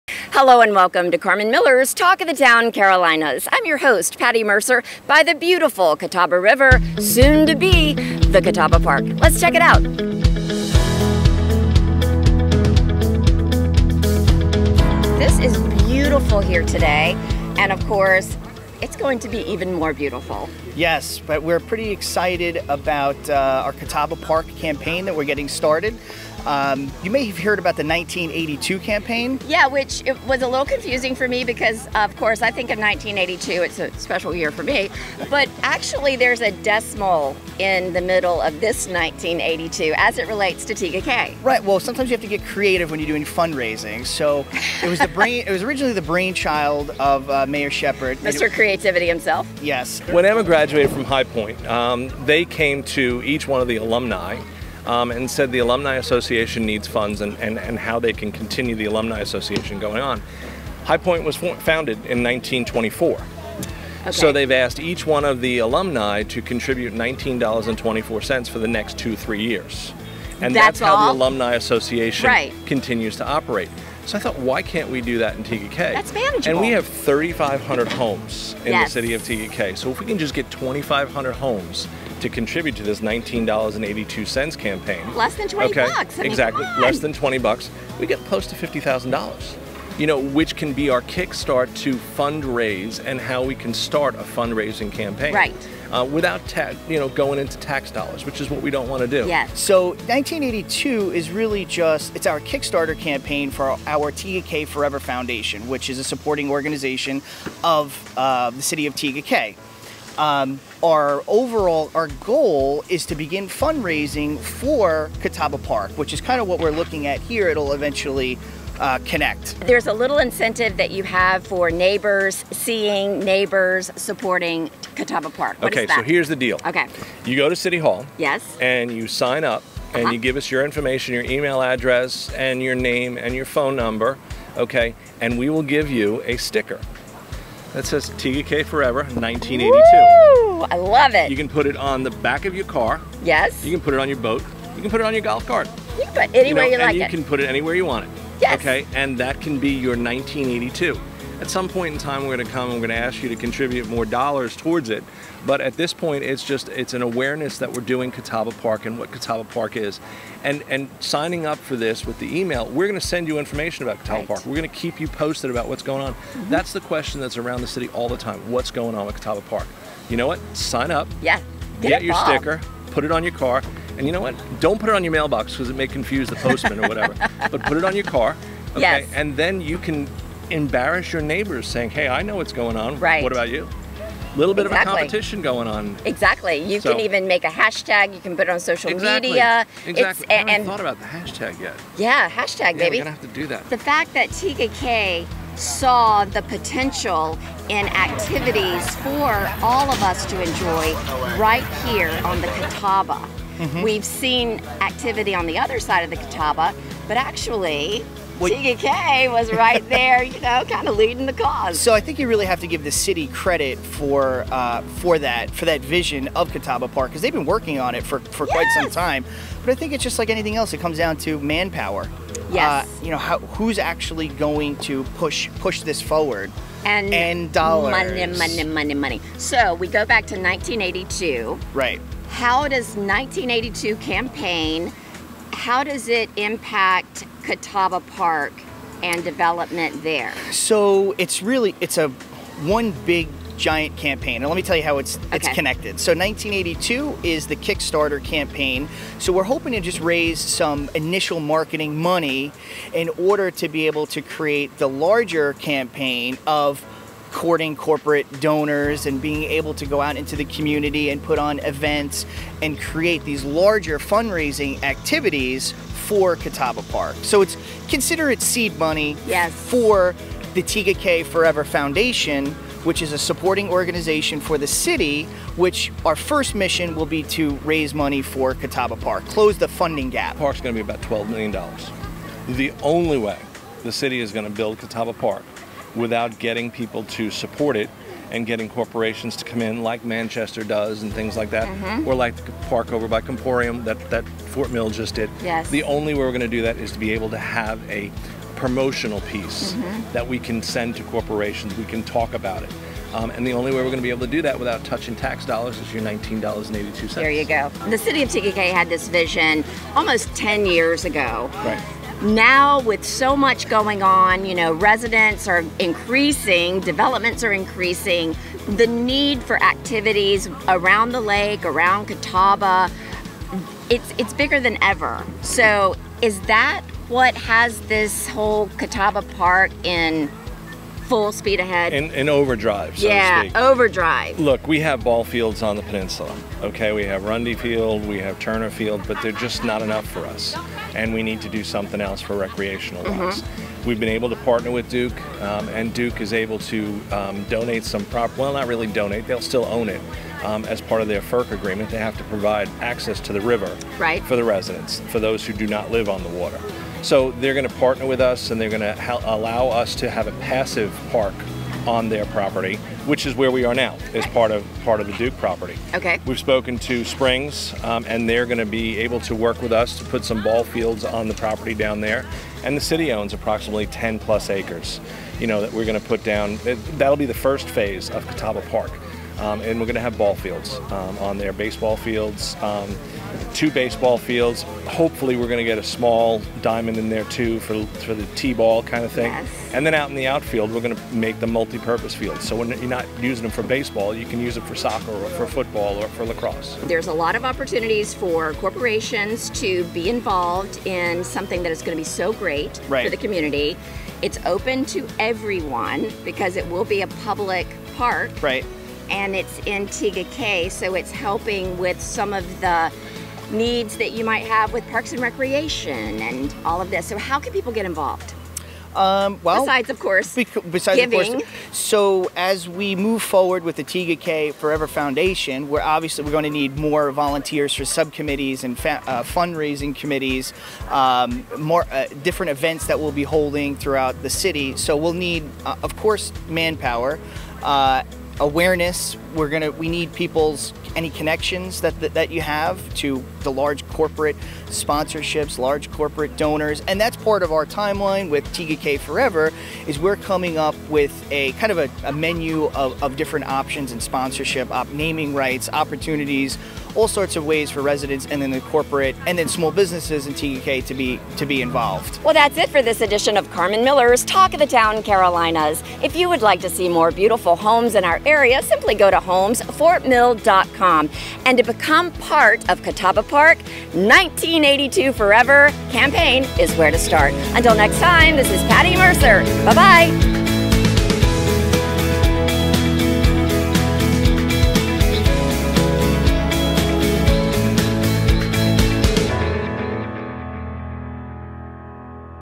1982 to Forever Tega Cay Catawba Park Fundraiser
​What is the Tega Cay 1982 campaign, the Forever Foundation and where is Catawba Park? Talk of the Town Carolinas host Patti Mercer interviewed the Forever Foundation Board Chair Anthony Ruffalo and Tega Cay Mayor George Sheppard to find out more about the 1982 Forever fundraising campaign supporting Catawba Park.

The City of Tega Cay was born over 35 years ago and in celebration of the city’s birth and keeping with the tradition of planned growth, Tega Cay has announced the next phase of Catawba Park. Catawba Park is envisioned to be the preeminent recreational destination for individuals of all ages. To kick start the fundraising efforts and drive awareness for Catawba Park, the Forever Foundation started the 1982 fundraising campaign.

1.  As a  donor, your name will be featured on our 1982 donors’ page, mentioned in Tega Cay social media and Tega Cay  Rewind Newsletter. To donate please click here or stop by City Hall/Beach and Swim Center.

 2.  Spread the Word: Proudly display the 1982 “Destination Tags” on your cars, boats and golf cars to help drum up excitement about the 1982 campaign and jumpstart our Catawba Park fundraising efforts.

Talk of the Town Carolinas is a production by the Miller Realty Group LLC located at 7675 Tega Cay Dr. Tega Cay, SC 29708.